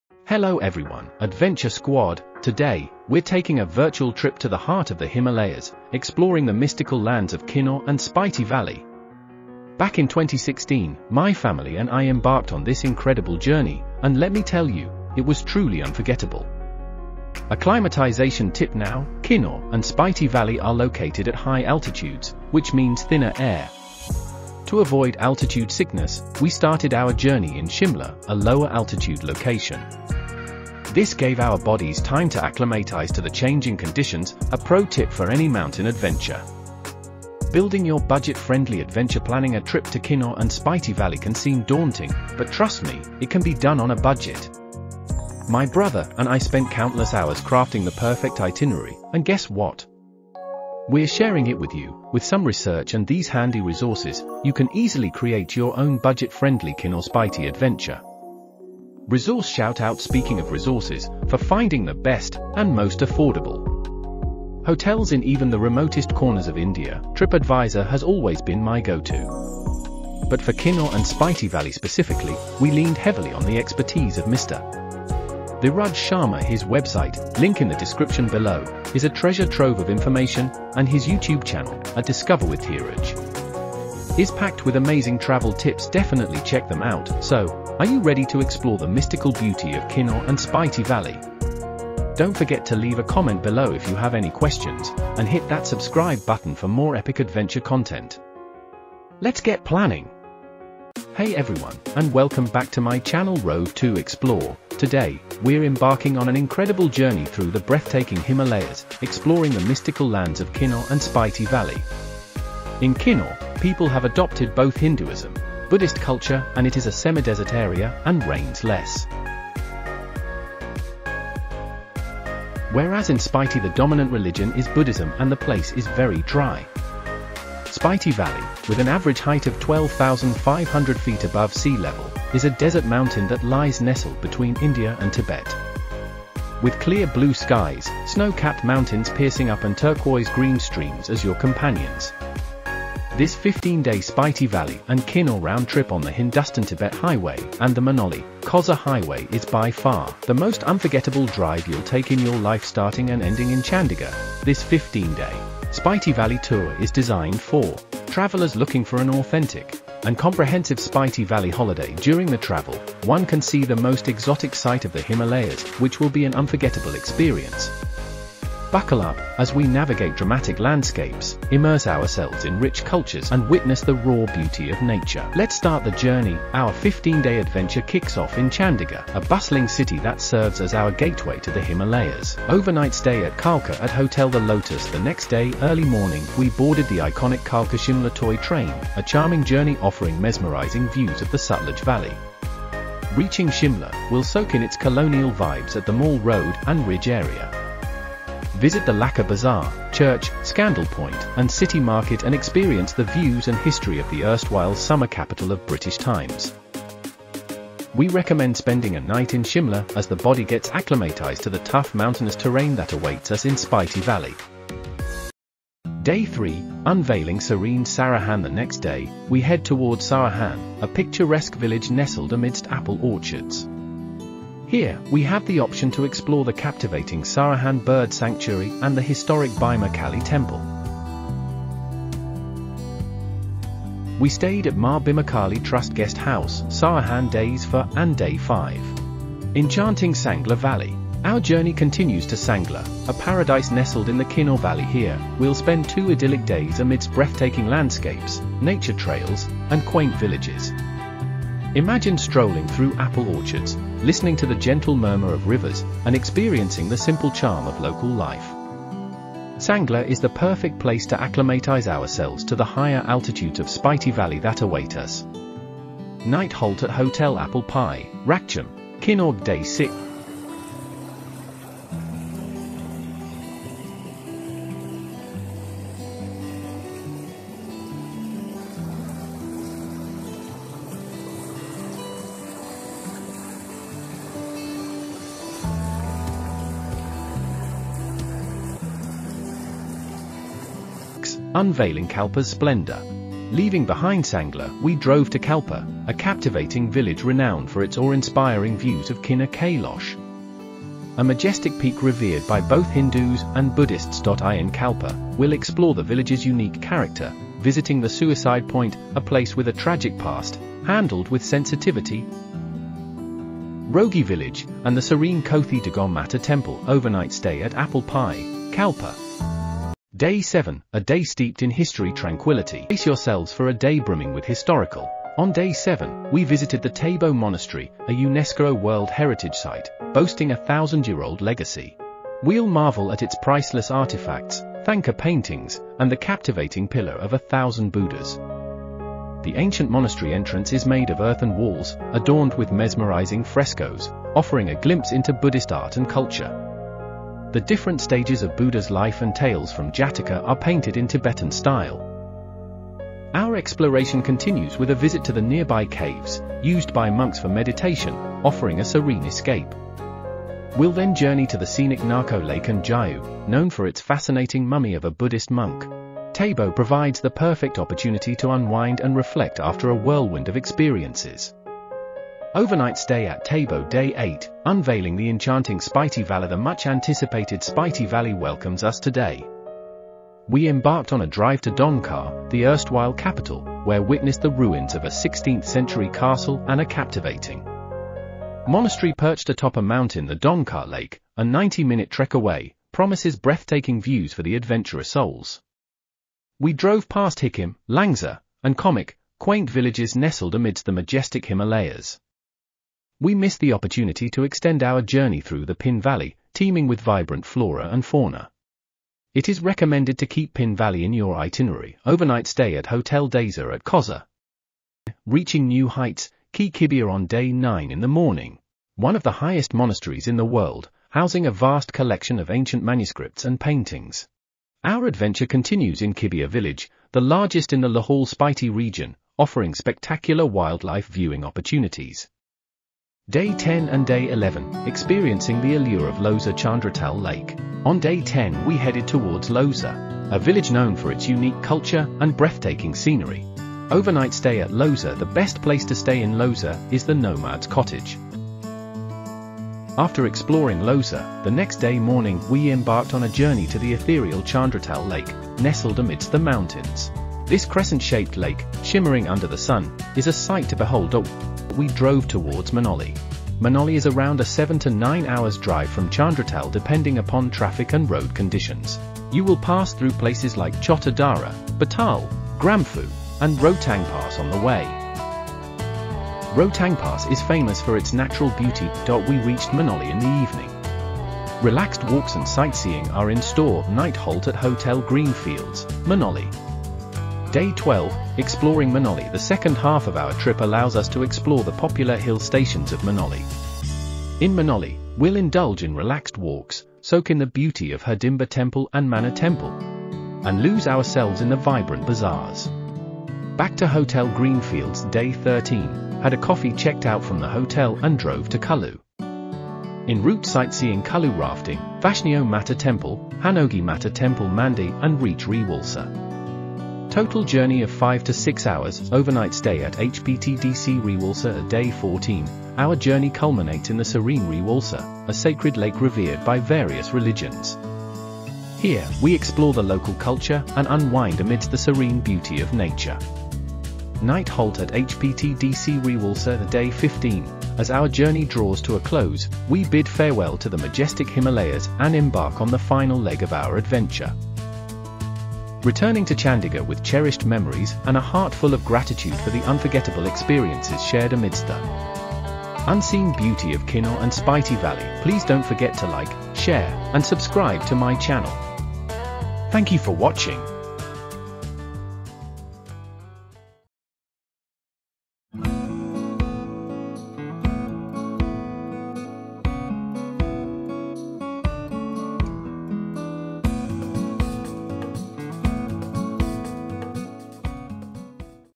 The Unseen Himalayas: A Journey Through Kinnaur-Spiti
Once in a lifetime road trip
Day 01:
Our adventure kicks off in Chandigarh, a bustling city that serves as our gateway to the Himalayas.
Day 02:
Experience the iconic Kalka-Shimla Toy Train, a charming journey that offers mesmerizing views of the Sutlej Valley. During your trip to Shimla, you can explore local sights like Mall Road and the Ridge area. Take a stroll through Lakkar Bazaar, visit the Church, Scandal Point, and city market, and learn about the history of the old summer capital of India during colonial British times. After a day of sightseeing, relax with an overnight stay in Shimla.
Day 03:
After breakfast check out from the hotel and drive to Sarahan. On the way, we’ll pass Wangtu Bridge and reach Sarahan in the evening. If early, we can see the Sarahan bird sanctuary and Bhim​a Kali temple. We stayed at Ma Bhimakali Trust Guest House, Sarahan.
Day 04:
Drive from Saharan to Sangla. We spend two days in Sangla enjoying the valley landscape, nature trails, apple orchards, and the simple way of life of local villagers. We can explore numerous small rivers flowing through the area and relax on the riverside. The time spent here will help us acclimatize to the higher altitude of Spiti Valley. Night Halt at Hotel Apple Pie, Rakcham, Kinnaur
Day 05:
Sangla local sightseeing Rakchham, Chitkul, Kamru Fort back to Hotel. Night Halt at Hotel Apple Pie, Rakcham, Sangla
Day 06:
After breakfast check out from the hotel and drive from Sangla to Kalpa. On arrival check into the hotel, Kalpa local sightseeing Kalpa village suicide point, Roghi village, Kothi Durga Mata Temple, and Back to Hotel. Night halt at Apple Pie, Kalpa
Day 07:
After breakfast checkout from the Hotel and Drive from Kalpa to Tabo. Visit Tabo Monastery is among the oldest Buddhist monasteries in India with a history dating back to 1000 years. Tabo Monastery is listed as a World Heritage Site by UNESCO. Soon after, we’ll visit caves in the mountains nearby which are used by monks for meditating. We next visit Nako Lake on the way and Giu with its mummy of a Buddhist monk. You can relax 1 day more in Tabo from a hectic schedule.
Day 08:
Checkout from the Hotel after breakfast and drive from Tabo to Kaza. We drive to Dhankar, the ancient capital of Spiti ruled by kings. Dhankar Lake is a 90-minute walk from here. We’ll drive towards Kaza, the capital of Spiti, and pass by Hikkim, Langza, and Koumik. Koumik whose main attraction is Lang this Natural Fossil Center. We visit Koumik monastery, one of the highest monasteries in the world as well as another ancient monastery near Hikkim village which was destroyed in an earthquake in 1975. Keep a water bottle and food with you. Carry UV-protected Sunglass with you throughout the entire tour. Visit Kaza market. Night Halt at Kaza. Hotel Deyzor at Kaza, Spiti valley
Day 09:
Next morning, we’ll drive on Kibber Road to visit Ki Monastery located at a height of 4120 meters. Next, we drive to Kibber village (4250 meters) which is among the biggest villages in Lahaul Spiti region. This is a great place for clicking pictures of wildlife. Night halt at Hotel Deyzor, Kaza.
Day 10:
After breakfast drive from Kaza to Losar. Local sightseeing at Losar. Night halt at The Nomad's Cottage or Home Stay, Losar. The best place to stay in Losar is The Nomad's Cottage.
Day 11:
Early morning drive from Losar to Chandratal Lake. After a visit to Chandratal Lake drive to Manali.  We’ll pass through places like Chotta Darrha, Batal, Gramphu, and Rohtang Pass on the way. Night halt at Manali at Hotel GreenFields, Manali.
Day 12:
Local sightseeing at Manali. Visit bazaars and landmarks like Hadimba Temple and Manu Temple. In Indian mythology, Manu is considered the first human to inhabit Earth. Night Halt at Hotel GreenFields, Manali,
Day 13:
Check out from the Hotel and drive to Kullu. In route sightseeing Kullu Rafting, Vashnio Mata Temple, Hanogi Mata Temple Mandi and Reach Rewalsar. Total journey of 5 to 6 hours. Night Halt at HPTDC Rewalsar
Day 14:
After breakfast went for local sightseeing in Rewalsar. Back to HPTDC Rewalsar Hotel. Night halt at HPTDC
Day 15:
After breakfast check out from Hotel and Drive from Rewalsar to Chandigarh. As our journey draws to a close, we bid farewell to the majestic Himalayas and embark on the final leg of our adventure, returning to Chandigarh with cherished memories and a heart full of gratitude for the unforgettable experiences shared amidst the unseen beauty of Kinnaur and Spiti Valley. Night halt at Chandigarh.

The entire tour plan was done after Google from various websites. Special Thanks to discoverwithdheeraj, Tripadvisor and Booking.com